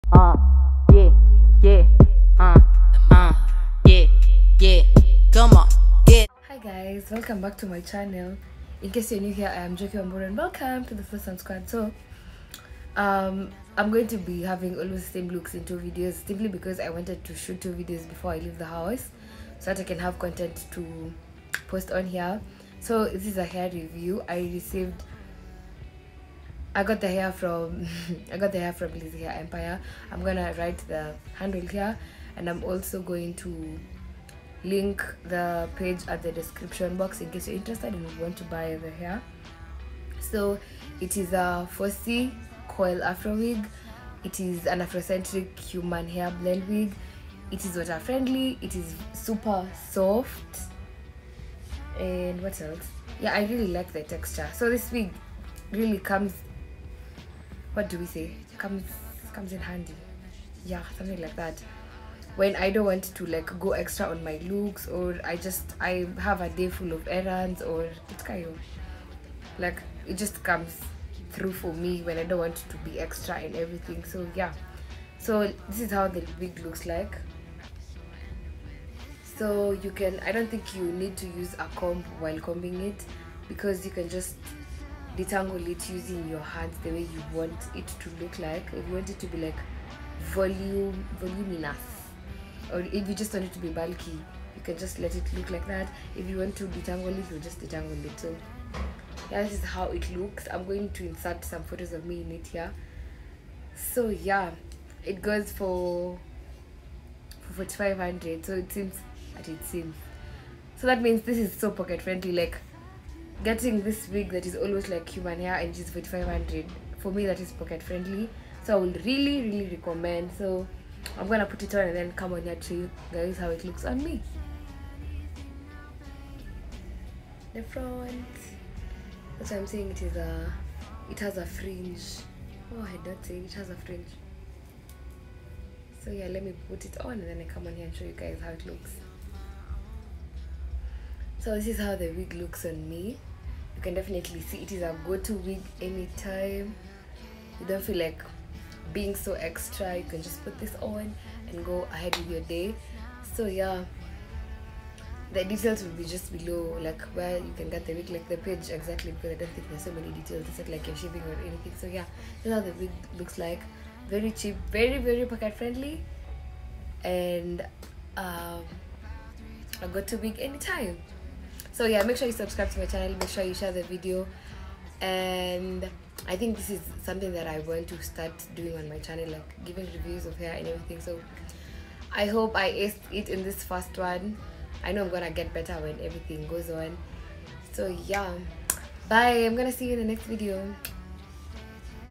The best Afrocentric humanhairblend wig Ft Lizzy_hairempire
A new week a new video idea
I’m planning to start doing hair reviews🤪so here’s my first one I hope y’all love it!!❤️

    💓~~Xoxo flawsome sq~~💓